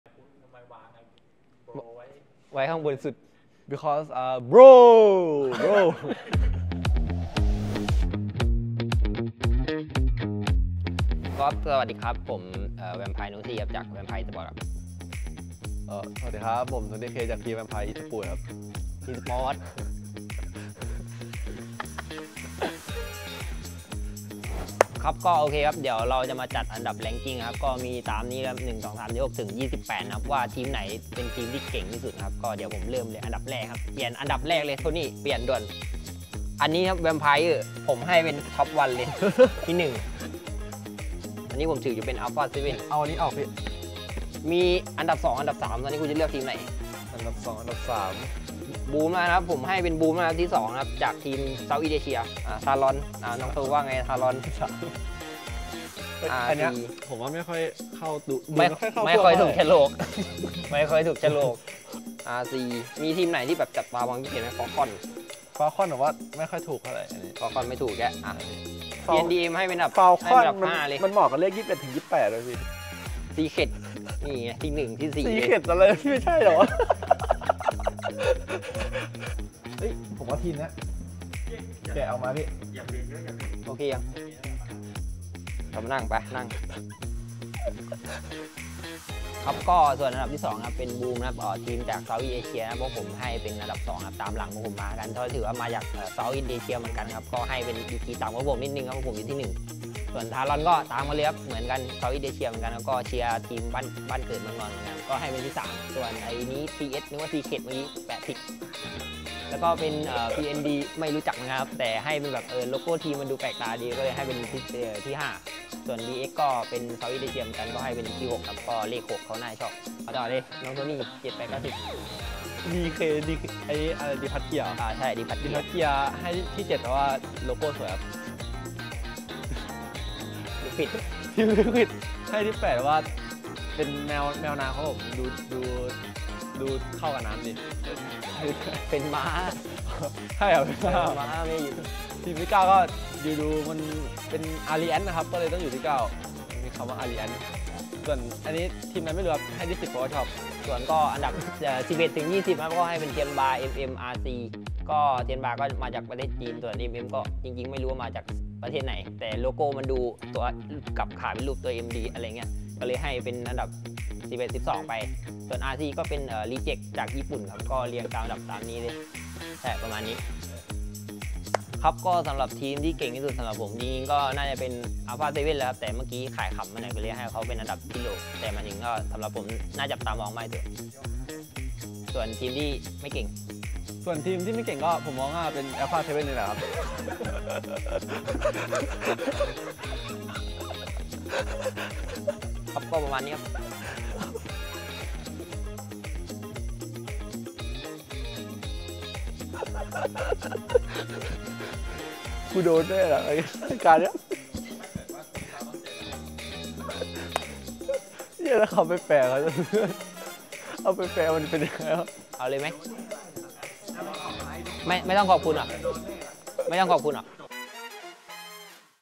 2024 PMWC POWER RANKINGS - EP02 | PUBG MOBILE ESPORTS
😂 Tonyk: "Harame Bro should be at the top, because Bro!!! Bro!!!"

Vampire Esports has revealed their power rankings! No rhyme or reason, just pure fun. 😆

Today's power rankings are full of twists and surprises. PUBG Mobile Esports fans, you won't want to miss this!

Let's also look forward to the exciting matches of the 2024 PMWC Group Stage Day 3 today.